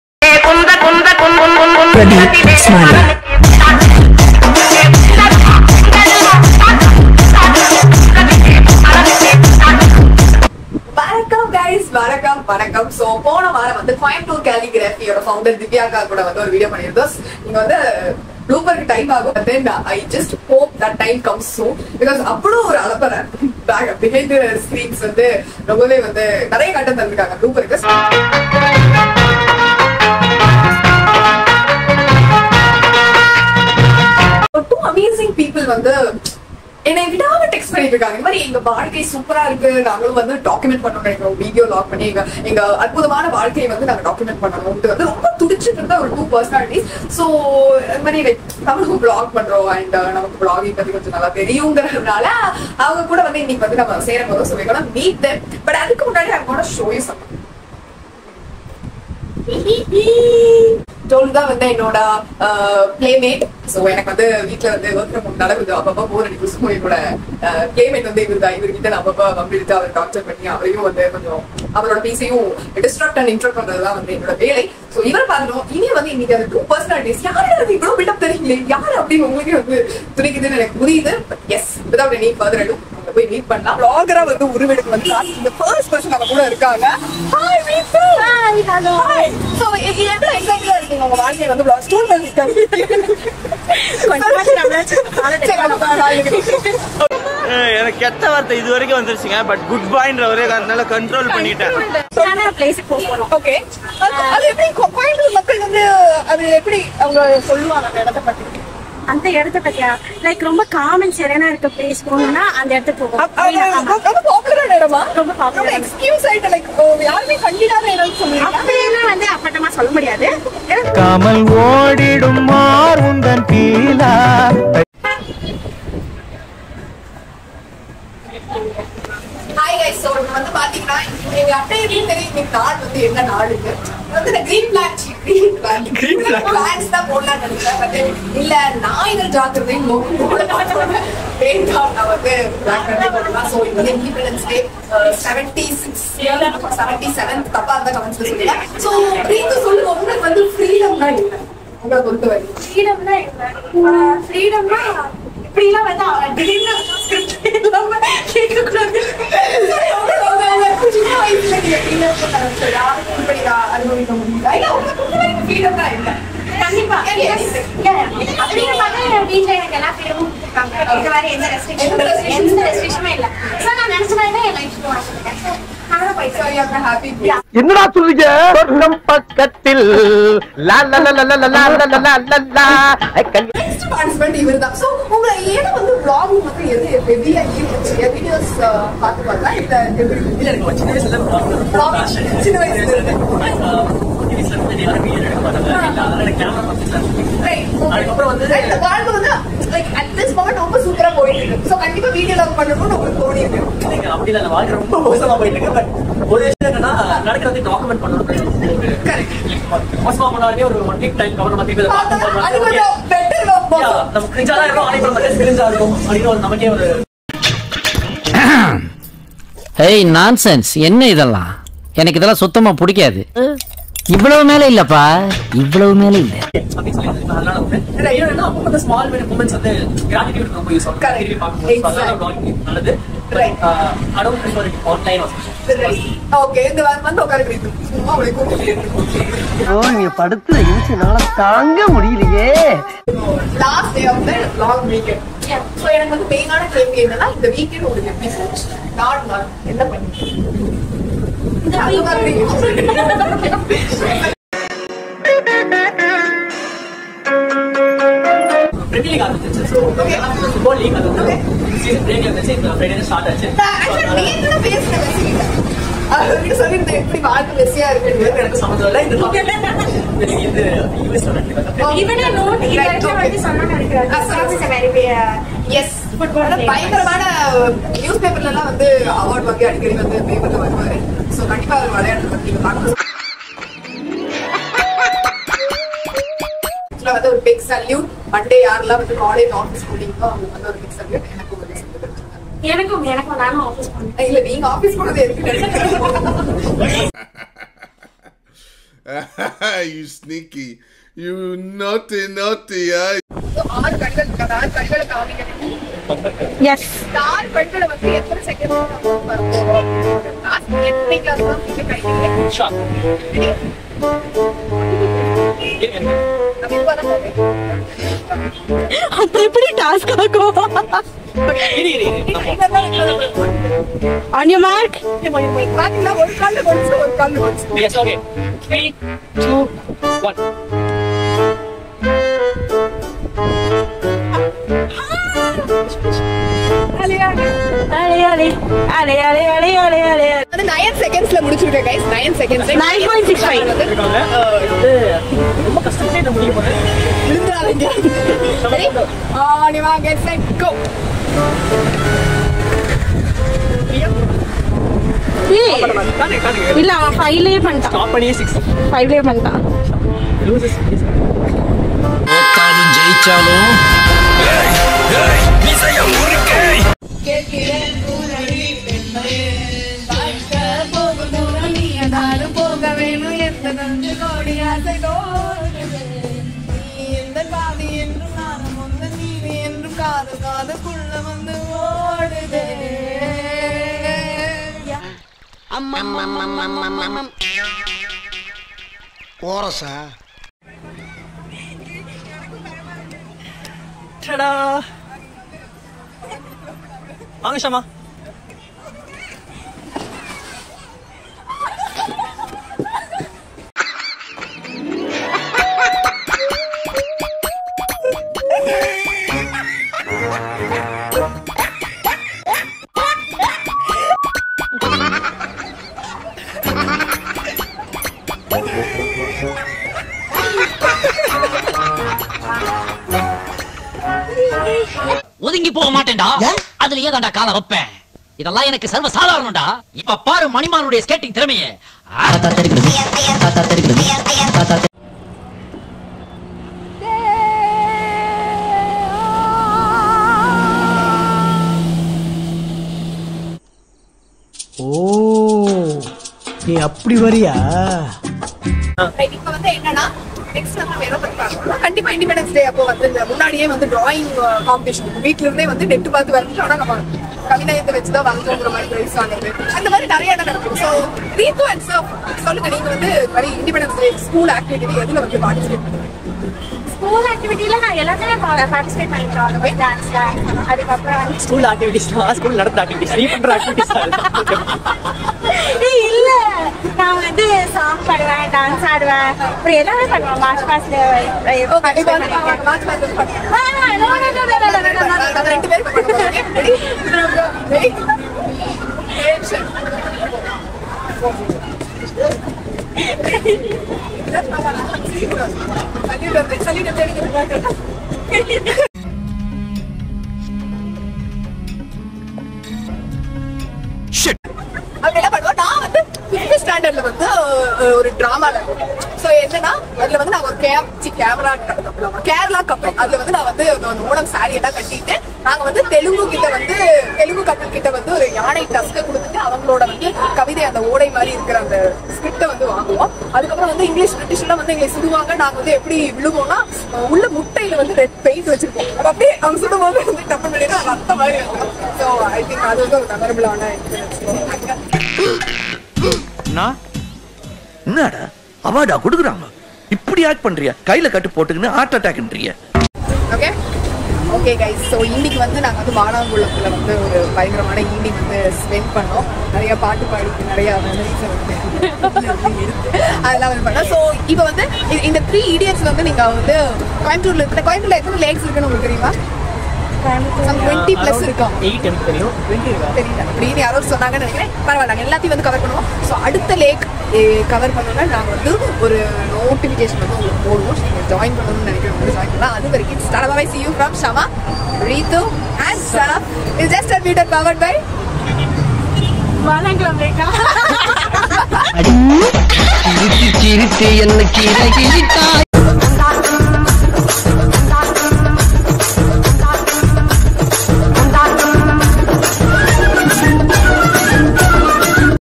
3 idiots | Valankulam Vlog | Jest Unmuted
Banter | Brains | Bangam

3 idiots - Valankulam Vlog - Stories of Jest Unmuted

Who knew hanging out with my juniors was all what I wanted.

Never felt like that though. Cuties are such kedis.

Talking and enjoying sunlight with them was therapy. I wish every week has a holiday like this,

It was a much, much awaited catchup and yes Coimbatore is so freaking beautiful on its own.

Are we set for the next lake visit cuties?